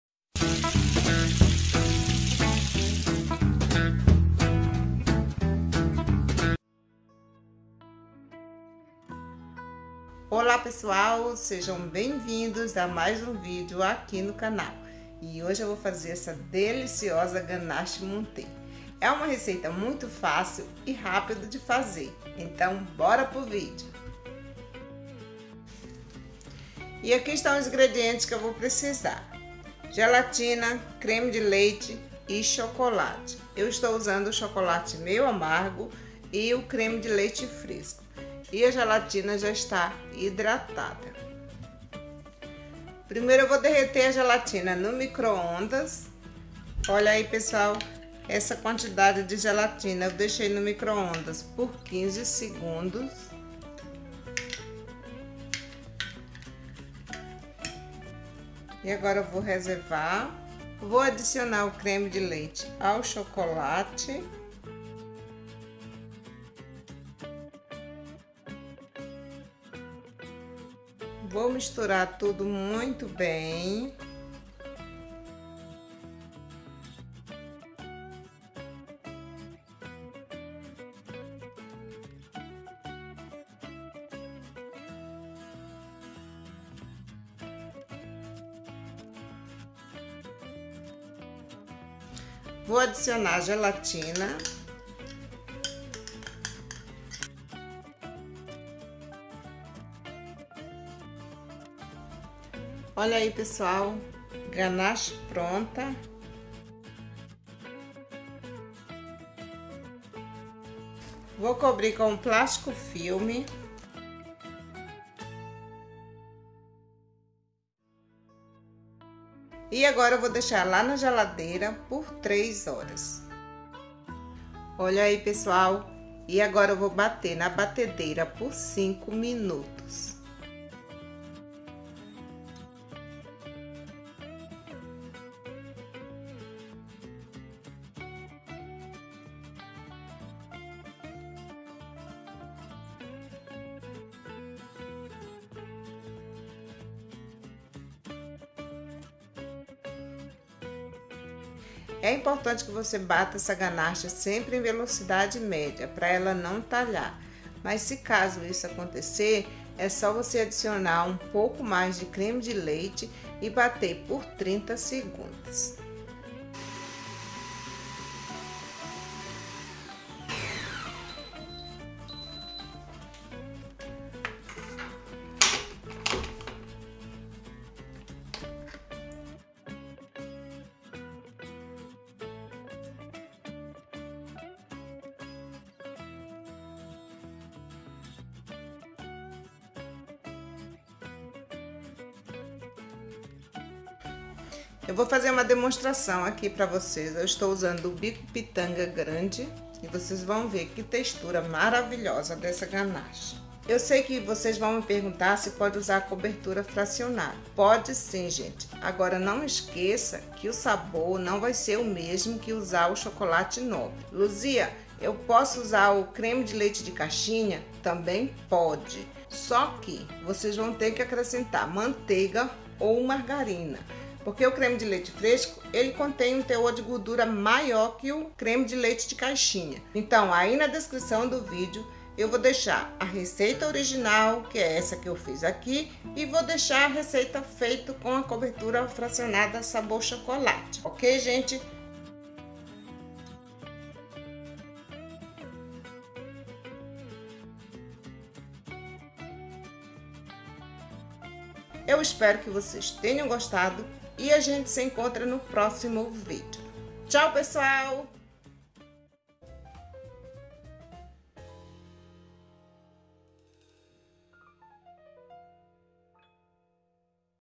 GANACHE MONTÉE - CHOCOLATE MEIO AMARGO - SUPER FÁCIL DE FAZER- Luzia Oliveira.
RECEITA:
Ganache para recheio cobertura, e decoração.
300 g de chocolate de sua referência derretido
300 g de creme de leite fresco ( leve ao fogo ou ao microondas para aquecer antes de colocar no chocolate )
5 g de gelatina hidratada e derretida

Receita feito com Cobertura Fracionada!
300 g de cobertura fracionada
300 g de creme de leite de caixinha ( leve  ao fogo ou ao microondas para  aquecer antes de colocar no chocolate.
2 colheres de sopa rasas de manteiga ou margarina derretida
5 gramas de gelatina hidratada e derretida.